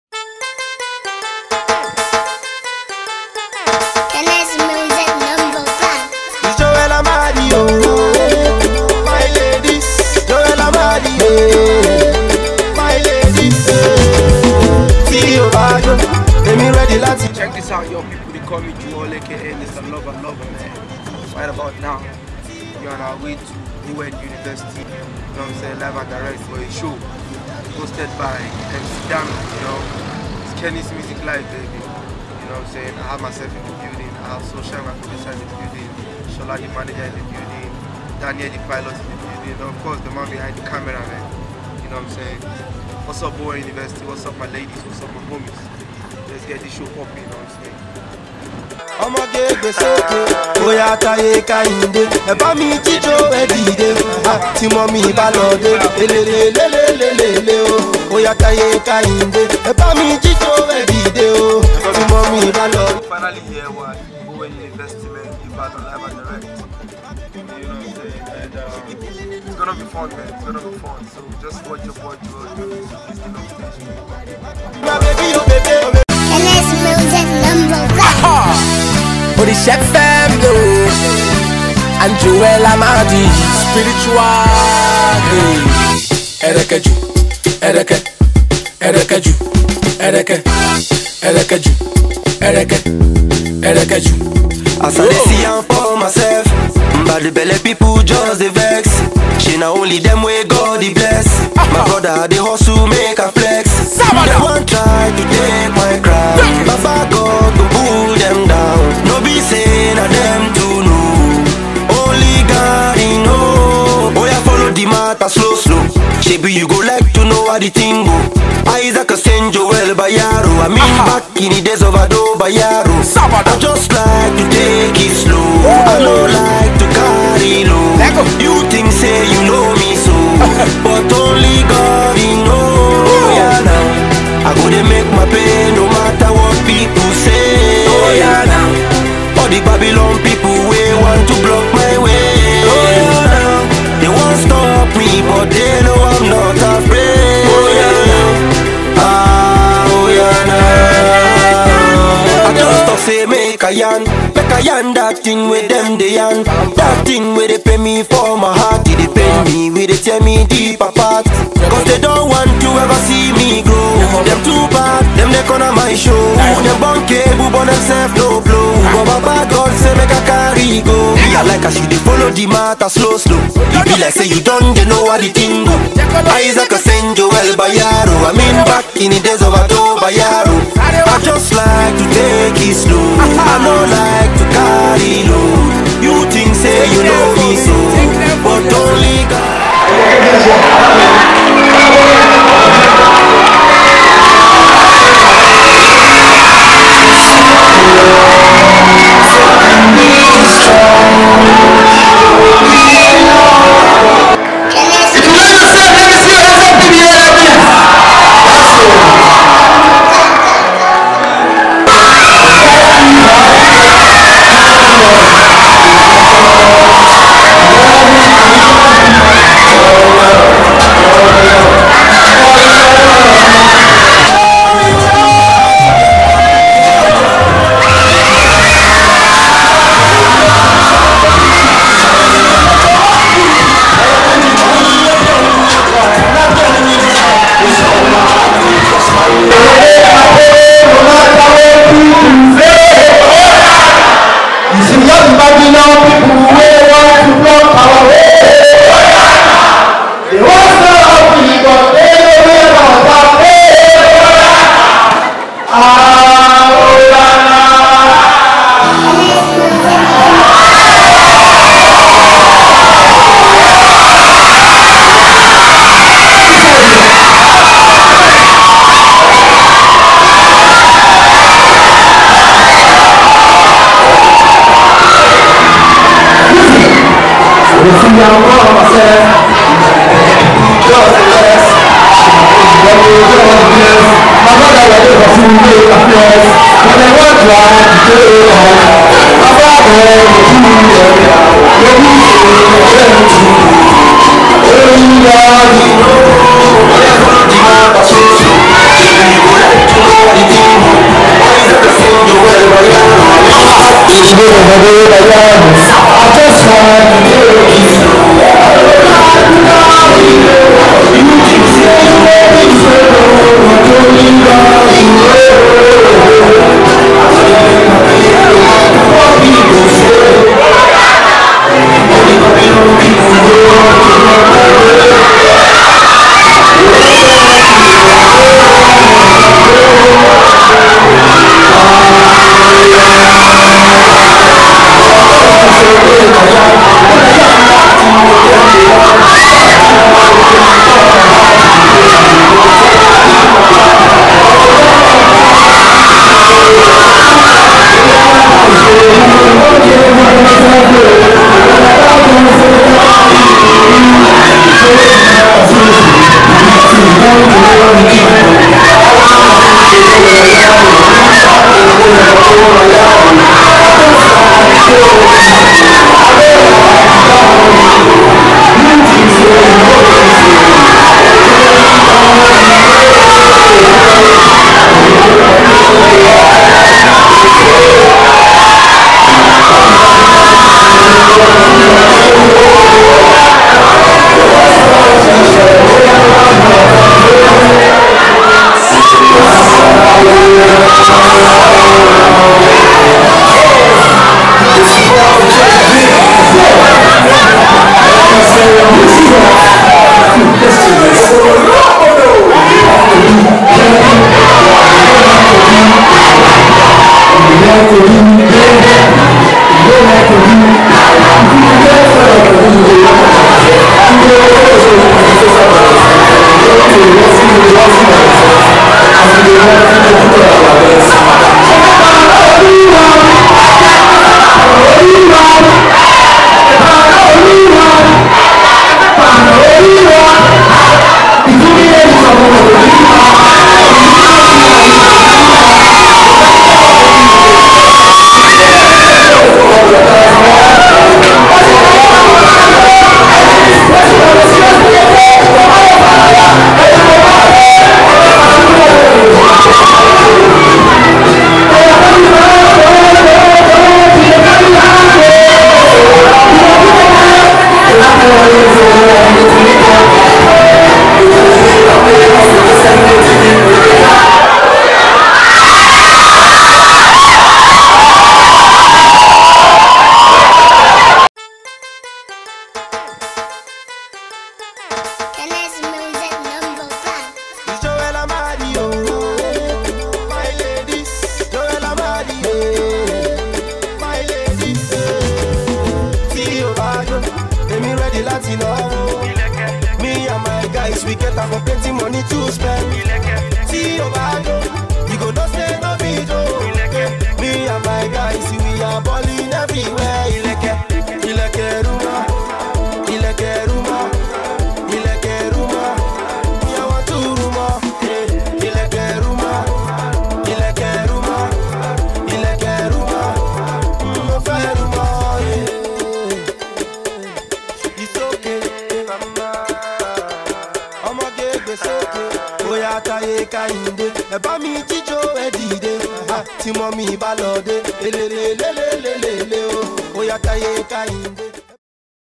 Joe EL On The Move Series "Episode 1" (Bowen University)
Kennis Music Presents the "Joe EL On the Move Series", This episodes are already recorded editing(s) of Joe EL's movements all around the globe.
The first edition is taken from his visit to Bowen University' a private university somewhere between the borders of Ibadan and Osun in Nigeria. The love is amazing, watch as he performs and drives the crowd crazy, its pure entertainment that will definitely last a while in the memory of the young students.
Watch, Enjoy and Share your views.